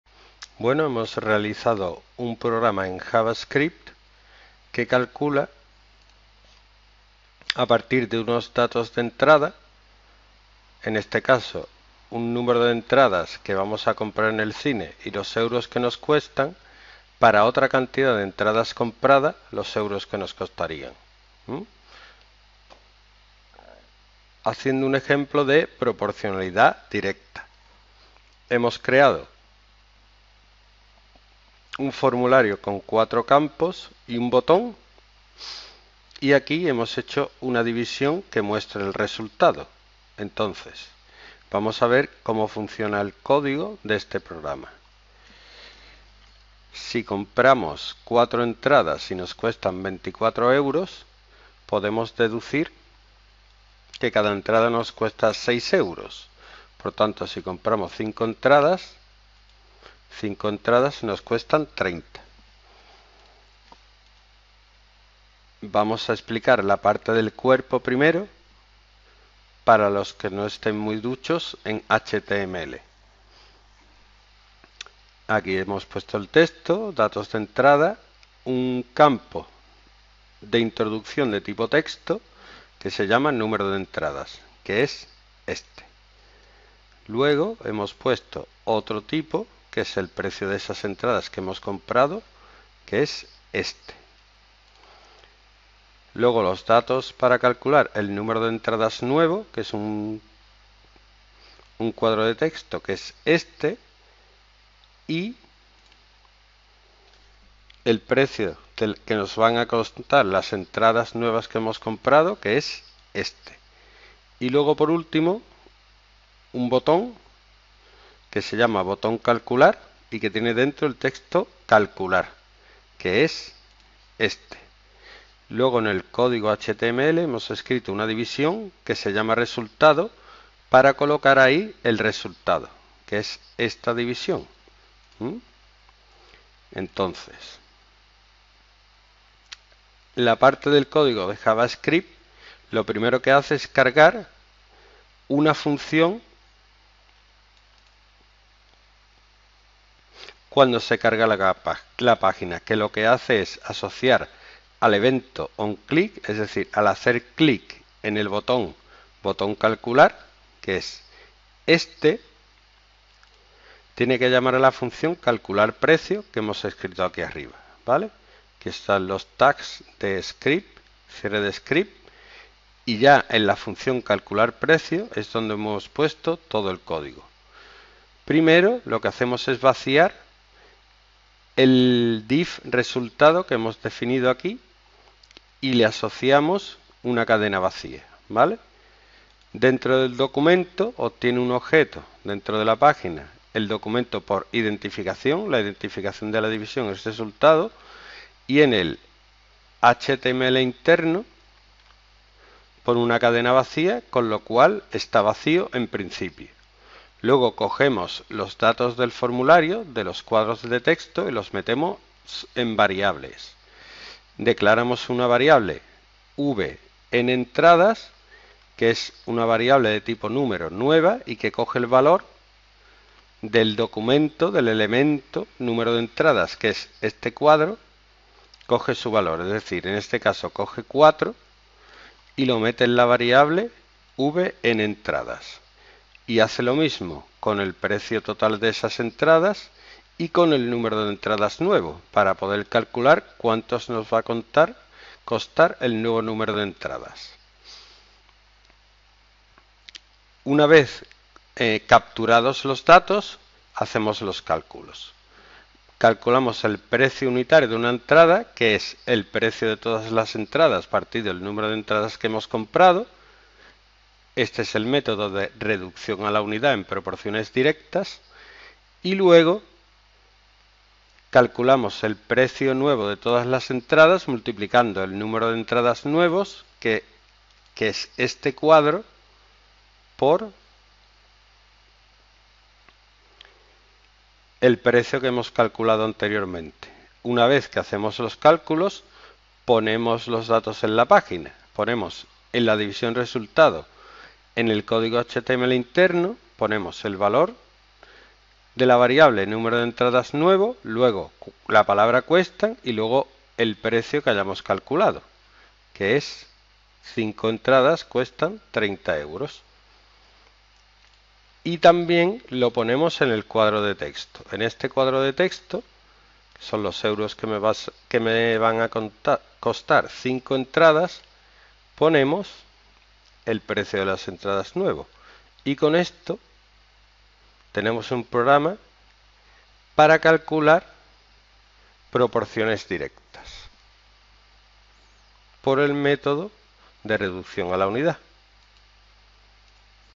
Proporcionalidad directa Javascript Academia Usero Estepona
Proporcionalidad directa en Javascript para calcular el precio que cuestan varias entradas al cine.